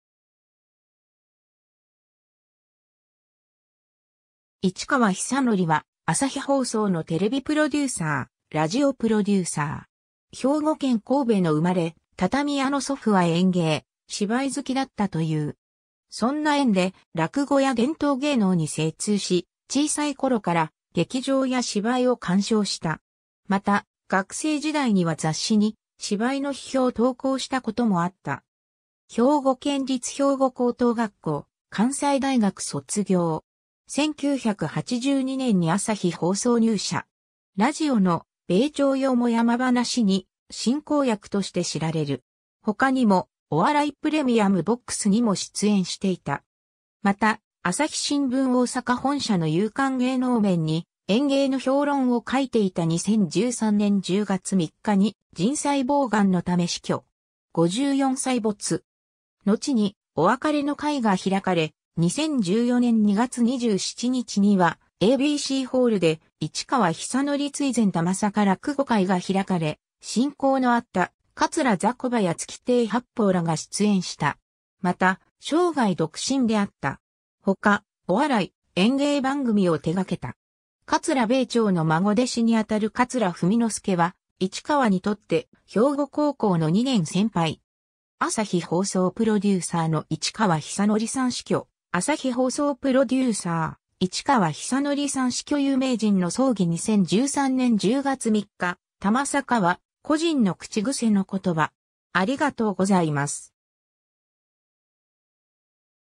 市川寿憲
市川 寿憲（いちかわ としのり、1959年1月17日 - 2013年10月3日）は朝日放送のテレビプロデューサー、ラジオプロデューサー。
 兵庫県神戸の生まれ、畳屋の祖父は演芸、芝居好きだったという。
そんな縁で落語や伝統芸能に精通し小さい頃から劇場や芝居を鑑賞した。
また学生時代には雑誌に芝居の批評を投稿したこともあった。
 兵庫県立兵庫高等学校、関西大学卒業。
1982年に朝日放送入社。
 ラジオの「米朝よもやま噺」に進行役（聞き手）として知られる。
他にも「お笑いプレミアム・ボックス」にも出演していた。
また、朝日新聞大阪本社の夕刊芸能面に演芸の評論を書いていた 2013年10月3日に腎細胞がんのため死去。
54歳没。
のちにお別れの会が開かれ、2014年2月27日にはABCホールで「市川寿憲追善 たまさか落語会」が開かれ親交のあった桂ざこばや月亭八方らが出演した。
また生涯独身であった。
 ほかお笑い、演芸番組を手がけた。
 ^ 桂米朝の孫弟子（桂枝雀の弟子）にあたる桂文之助は、市川にとって兵庫高校の2年先輩。
 ^ 朝日放送プロデューサーの市川寿憲さん死去（朝日新聞2013年10月3日　同12月15日閲覧） ^ 朝日放送プロデューサー・市川寿憲さん死去 有名人の葬儀 2013年10月3日 ^ 「たまさか」は故人の口癖の言葉。